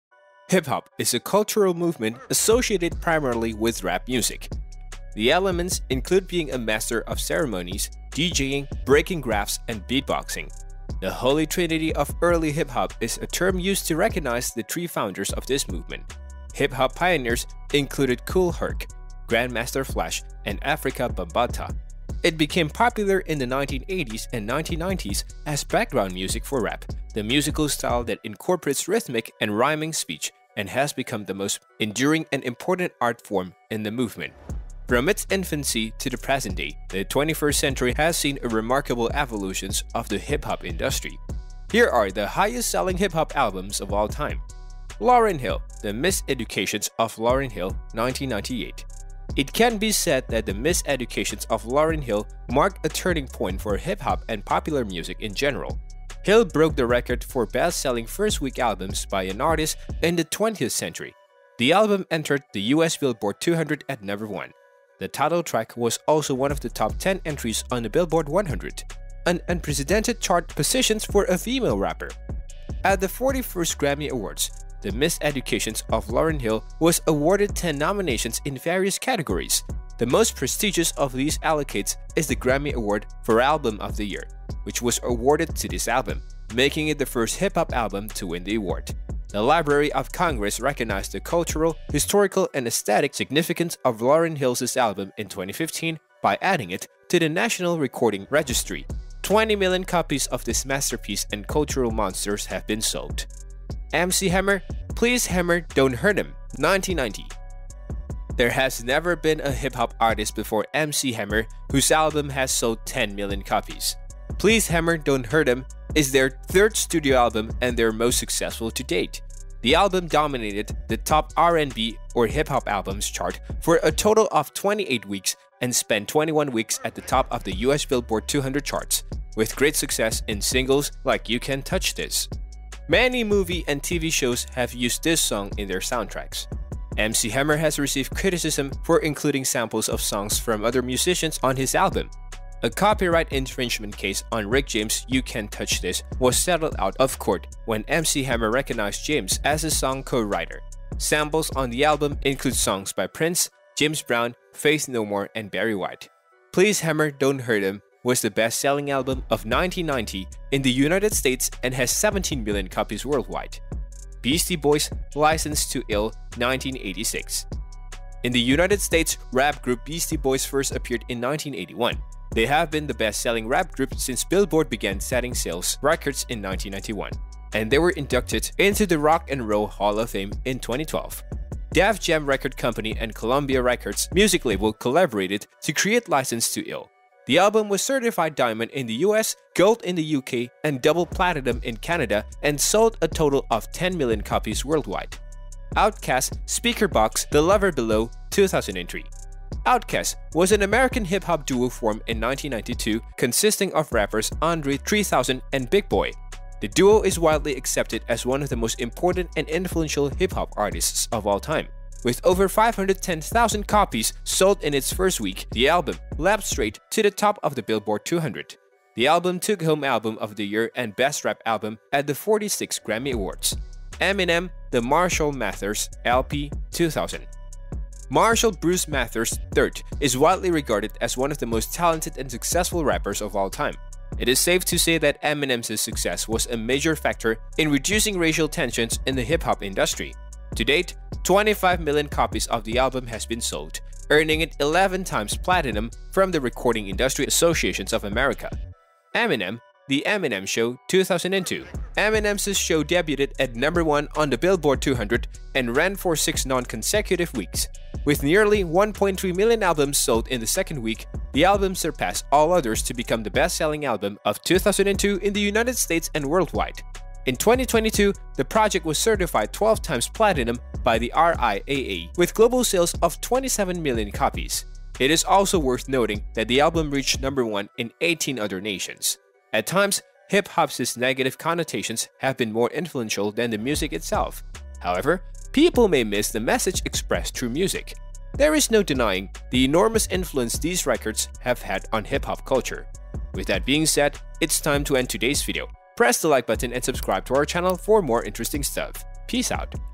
Highest Selling Hip Hop Albums of ALL TIME
In this video, we breakdown Highest Selling Hip Hop Albums of ALL TIME.